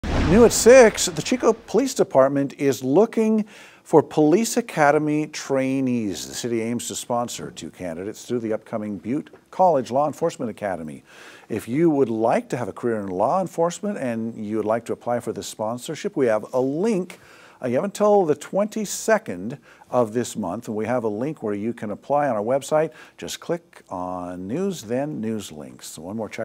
Chico Police Department is looking for new police academy trainees
You can apply on our website by just clicking on news links.